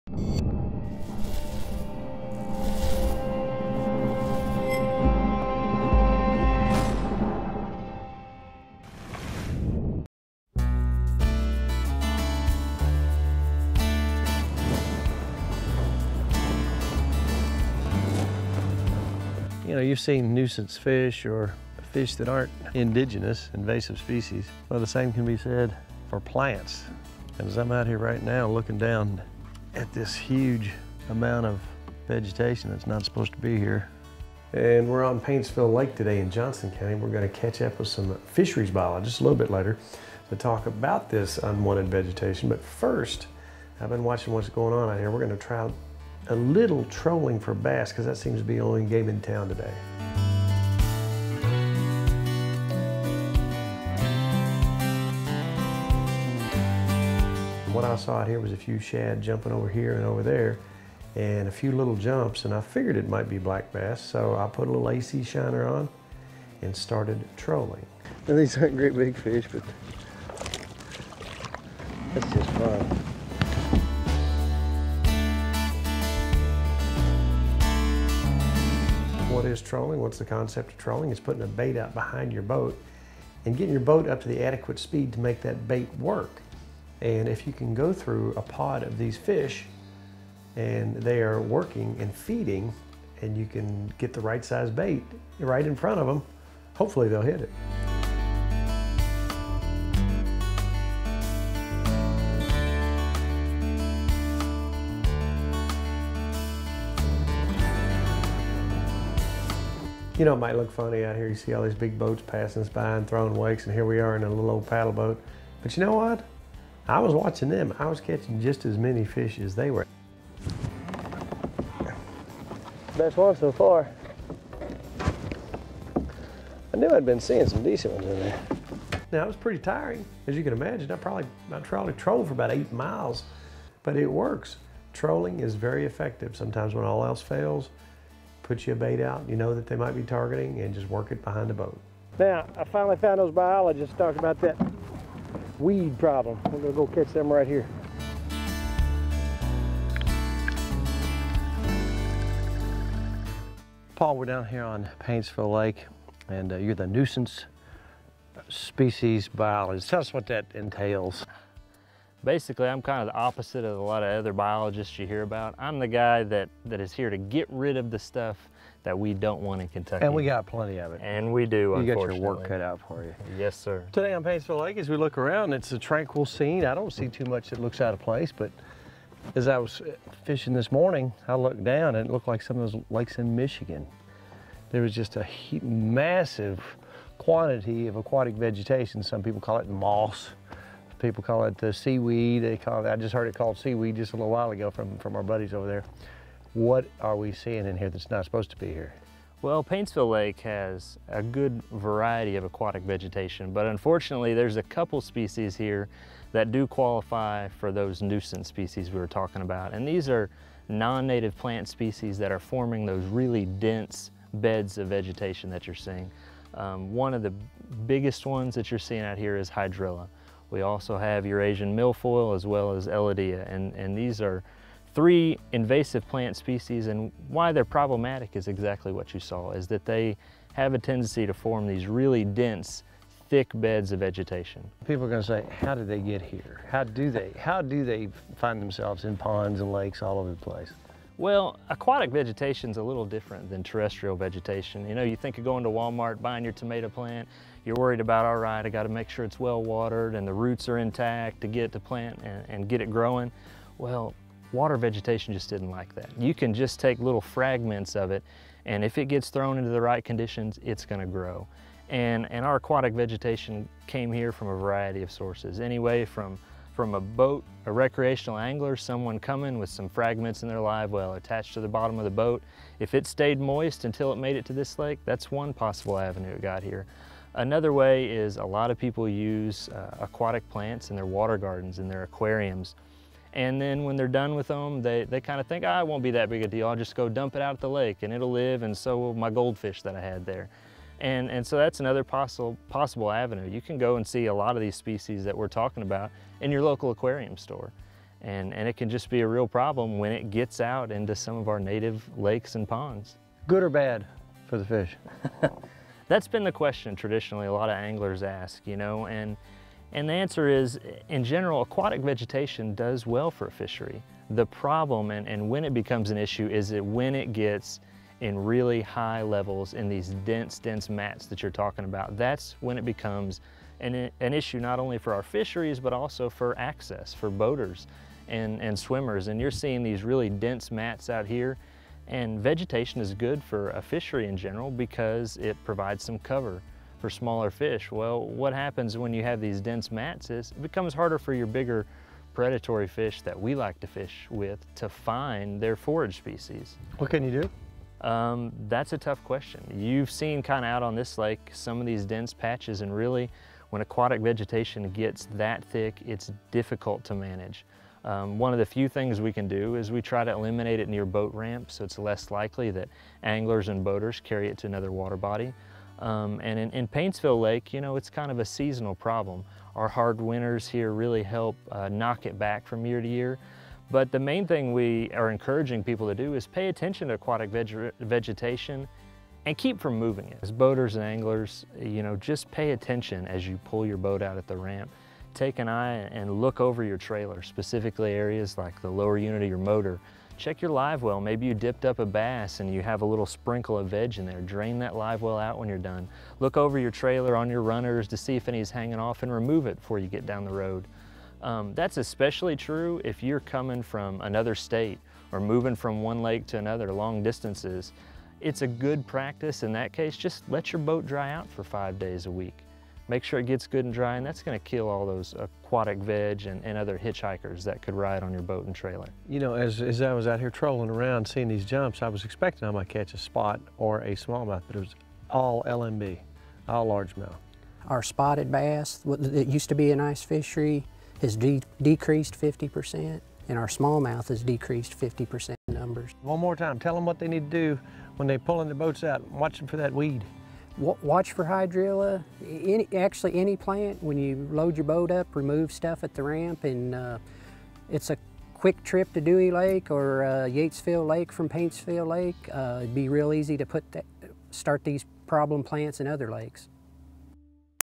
Paintsville Lake Invasive Vegetation
Tim trolls in his kayak for bass, then meets up with Nuisance Species Biologist Paul Wilkes to discuss nuisance vegetation in Kentucky lakes.  Watch this segment to learn how you can prevent spreading invasive species of vegetation from one lake to another.